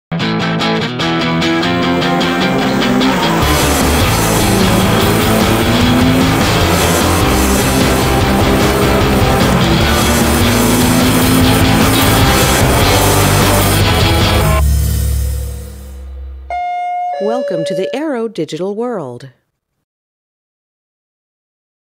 Arrow Lock Revolution Video #2
Promotional Video showcasing the new Revolution touchscreen electronic lock from Arrow